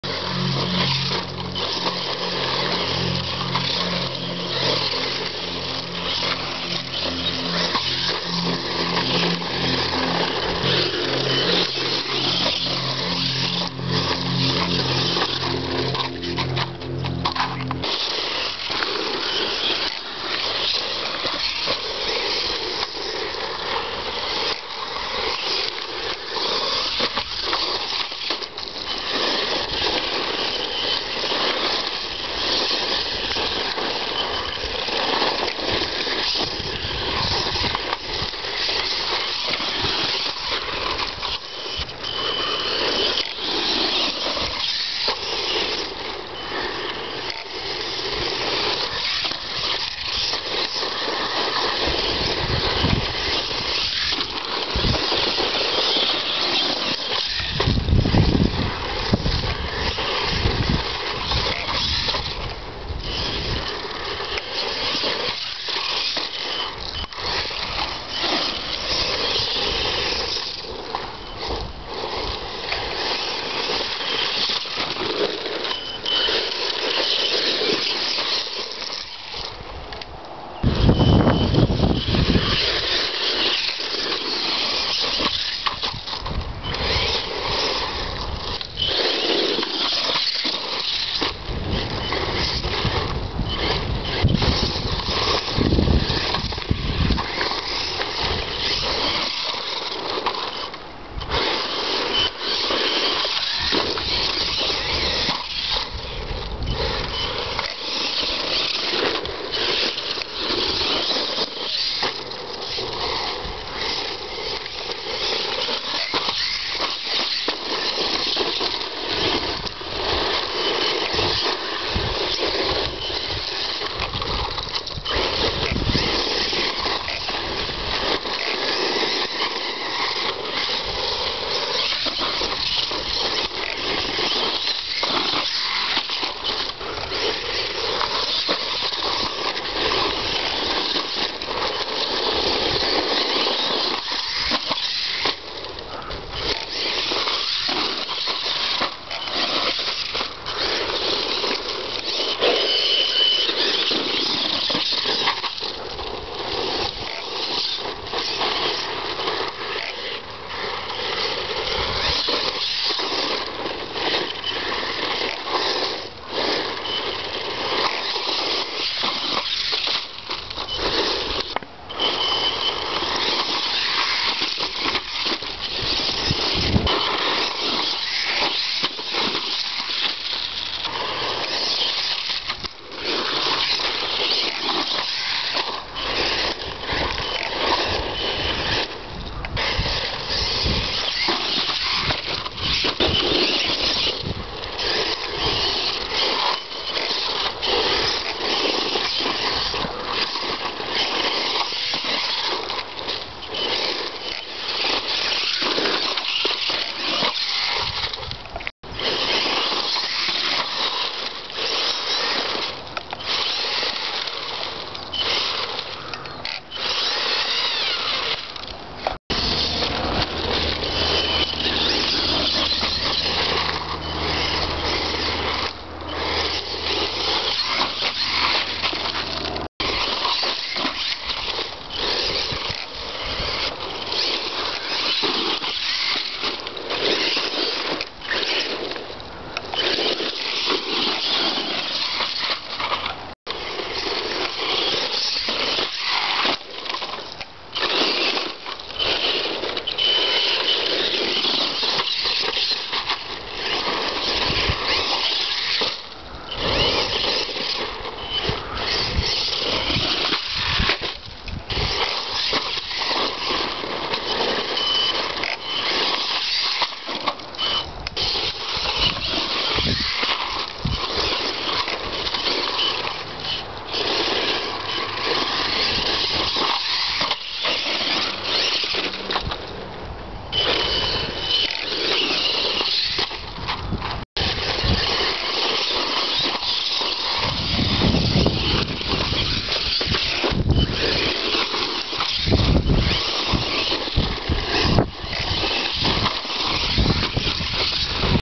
mini E-Revo catching air and stuff
This time I was running two 6-cell NIMH packs in parallel, which seemed to give measurably more than 2x runtime that of a single cell, and a little better grip on the loose dirt from the added weight.